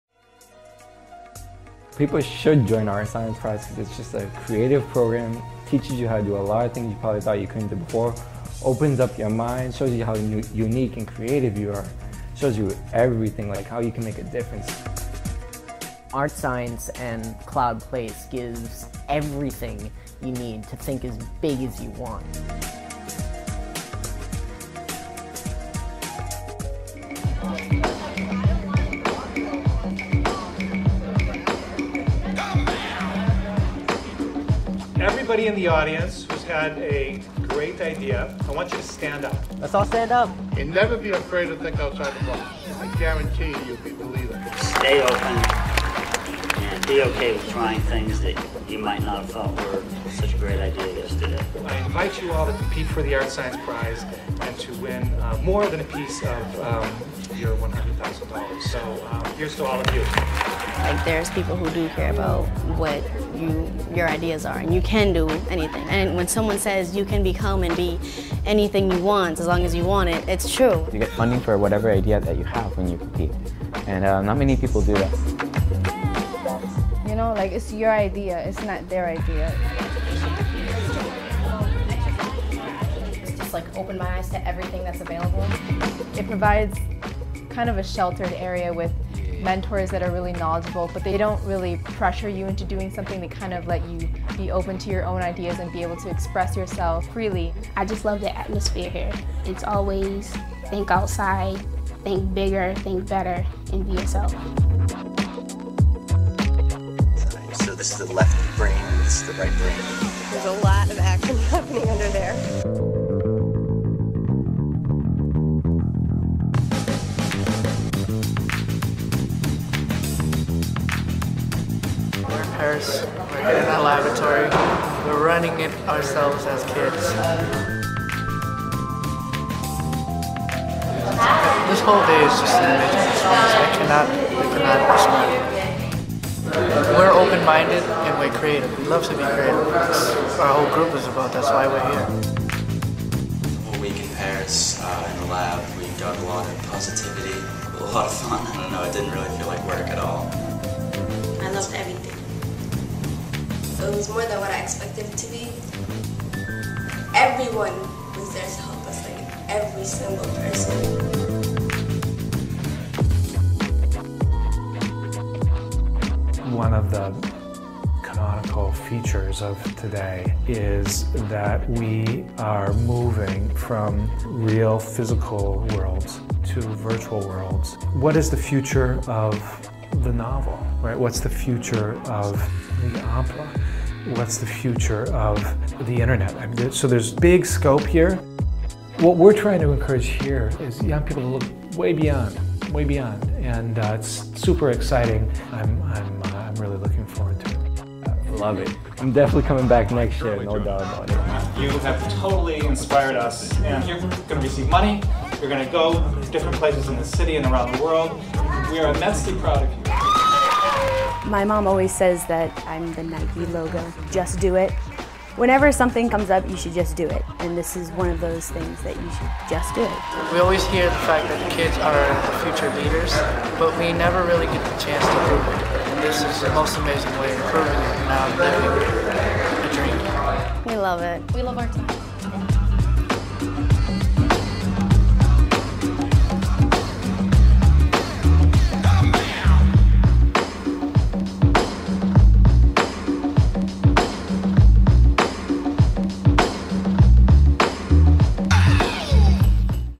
2011-2012 Boston ArtScience Prize
The ArtScience Prize is a year-long afterschool competition through which Boston Public High School students develop innovative art and design ideas informed by concepts at the frontiers of modern science.